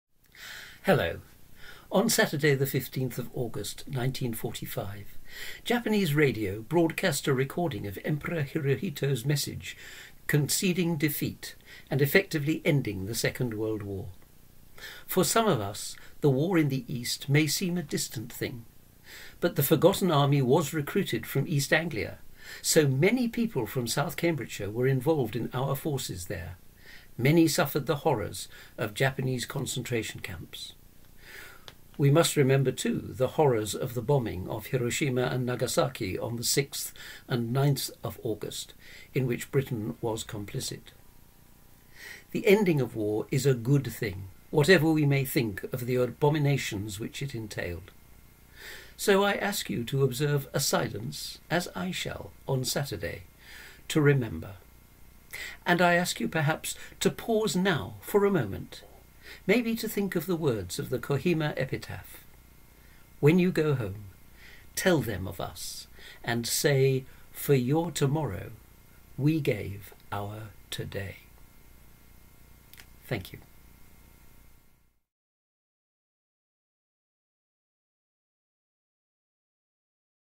Chairman's VJ Day message
A message from the Council Chairman, Cllr Douglas de Lacey, ahead of VJ Day 2020. Saturday 15 August 2020 is the 75th anniversary of VJ Day, marking both the surrender of Japan and the end of the Second World War. Find out more about this remembrance event